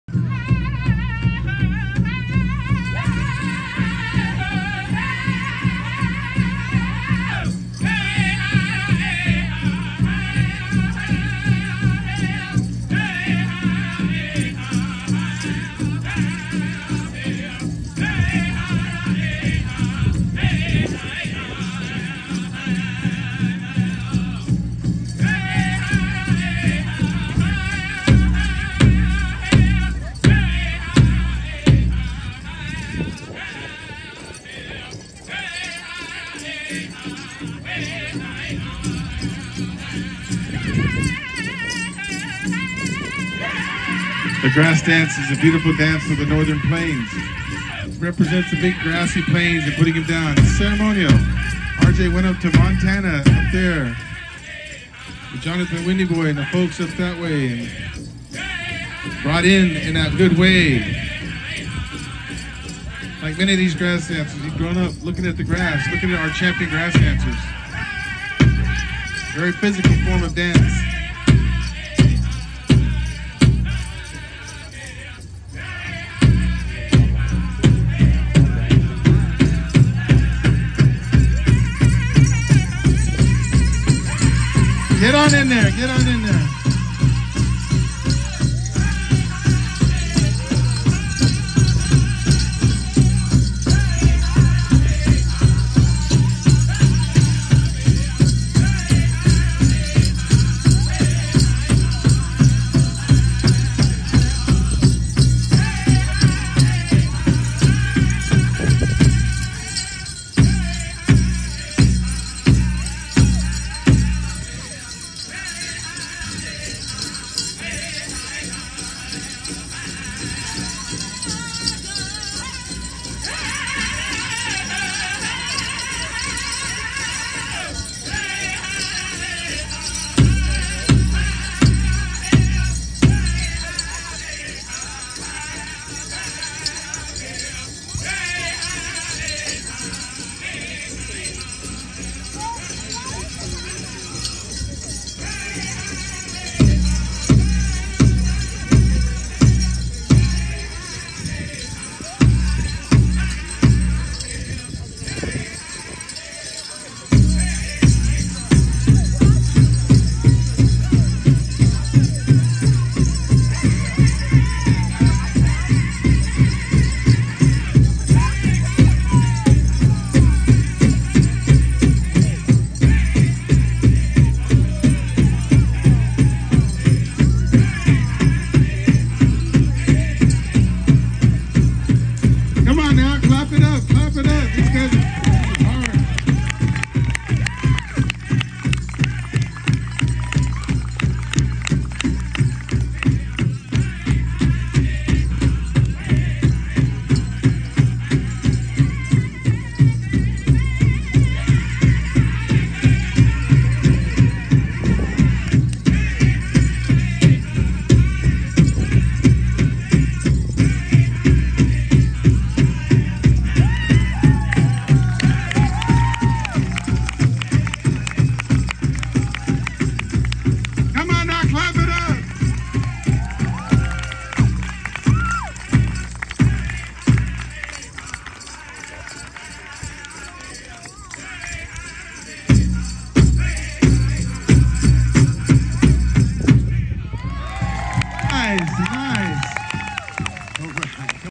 Grass Dance Contest @ Chumash Day Powwow
This is the grass dance contest from the Chumash Day Powwow in Malibu, CA.

The song is sung by Indian Hill. My cousins’ drum.